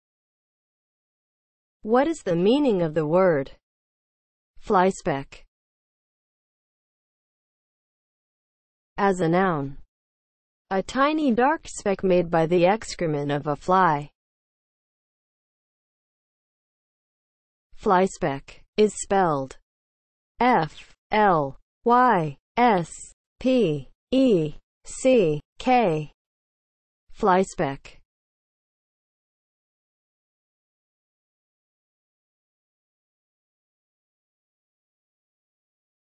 What is the meaning of the word FLYSPECK?
Definition and spelling of the word FLYSPECK.

(00:00) Intro
(00:08) As a noun
(00:16) Spelling

As a noun (00:08):
• a tiny dark speck made by the excrement of a fly